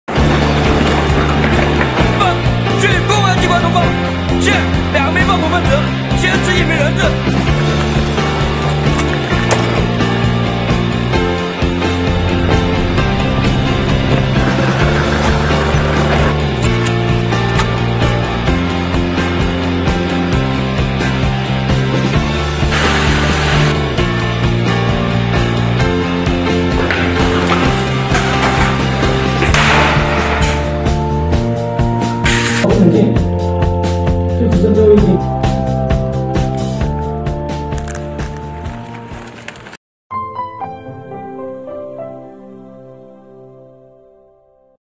直击云南武警反劫持演练：飞身索降 一枪毙敌
近日，武警云南总队德宏支队特战分队在实战化训练场，上演了一场不设预案的反劫持演练，全面锤炼特战队员在陌生复杂条件下的反恐制胜能力。